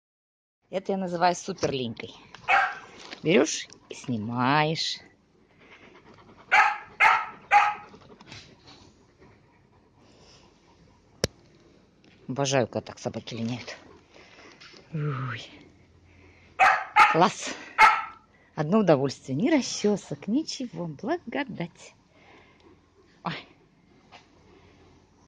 23 июня 2018 г.(2)Суперлинька
Гуля не жалеет пуха для мамы.